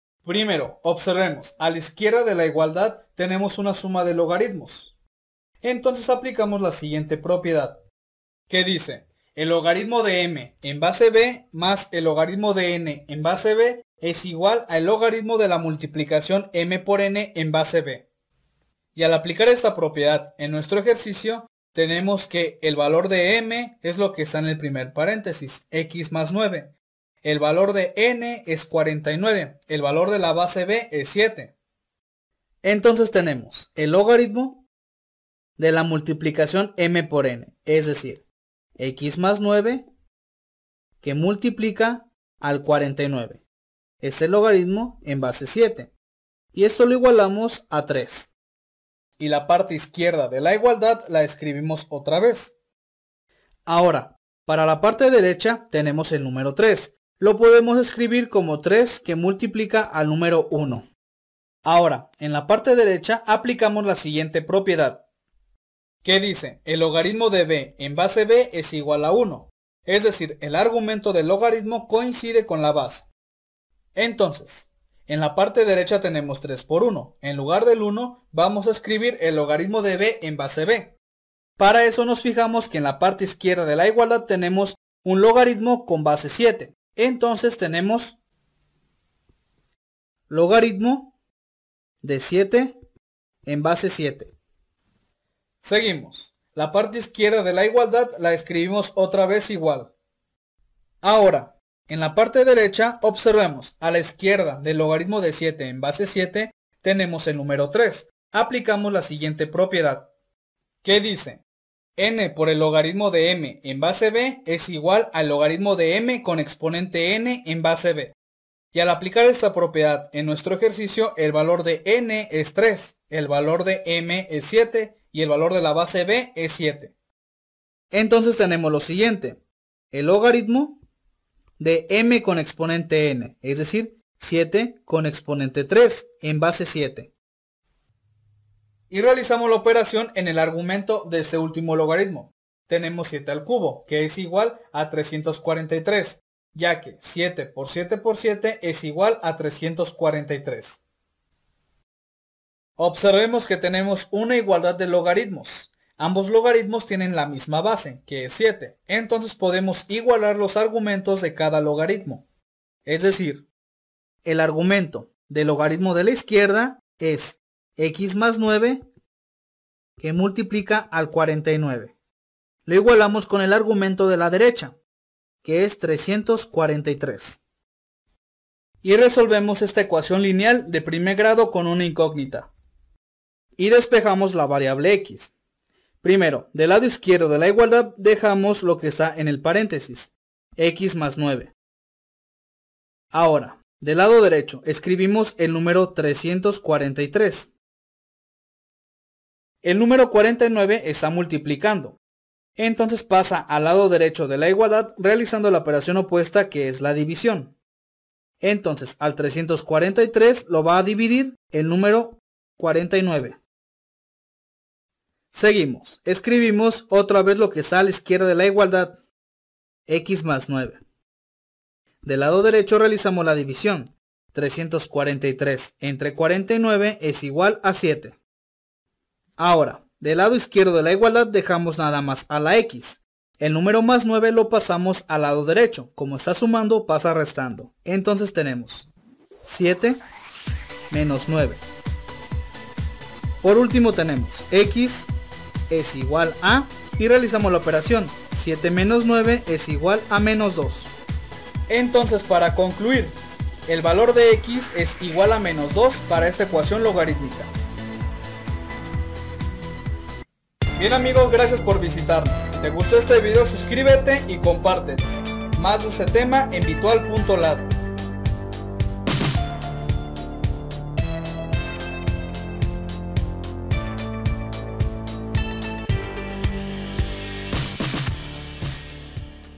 Ecuaciones Logarítmicas Usando Propiedades | Video 3 de 11
En este video aprenderás a resolver ecuaciones logarítmicas mediante sus propiedades.

Videos creados por